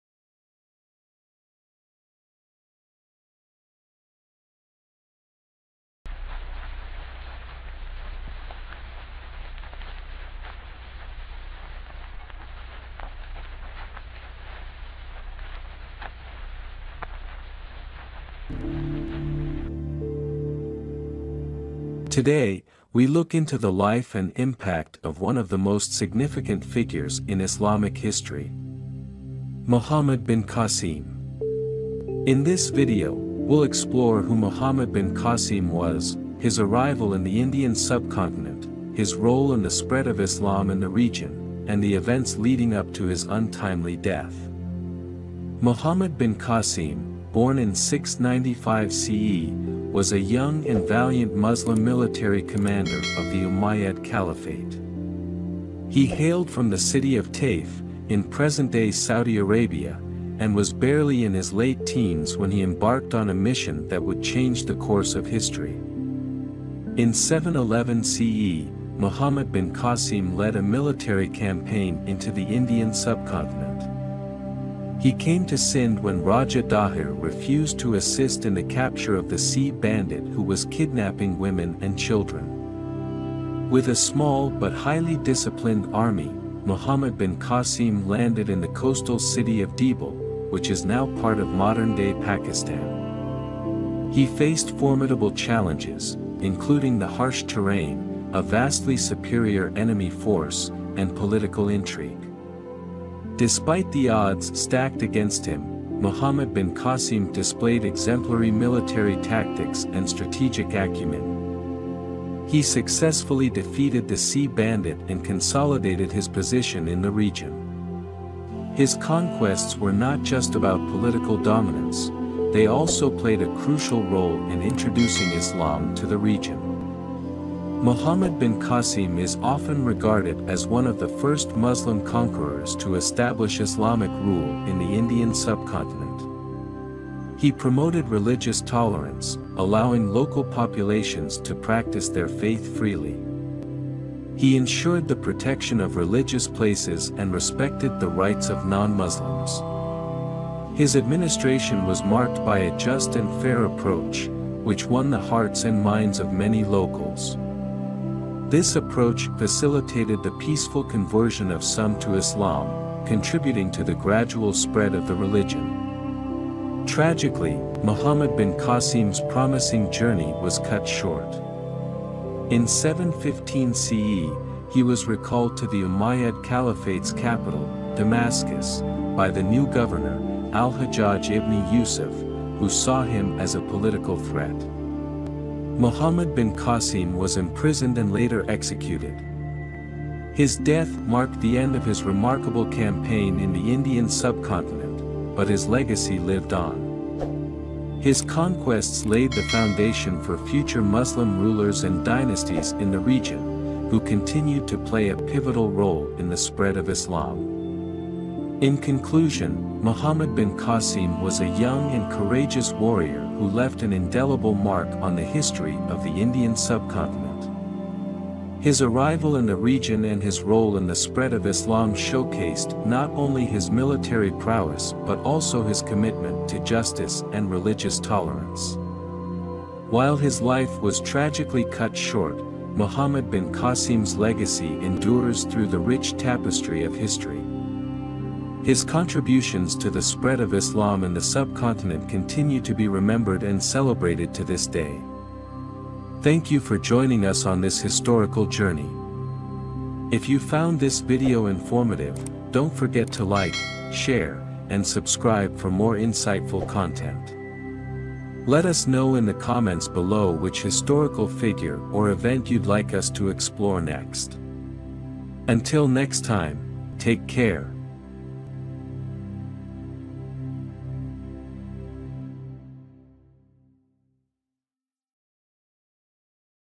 Muhammad Bin Qasim
His arrival in the Indian subcontinent, his role in the spread of Islam in the region, and the events leading up to his untimely death. His conquests were not just about political dominance; they also played a crucial role in introducing Islam to the region. He was a young and courageous warrior who left an indelible mark on the history of the Indian subcontinent.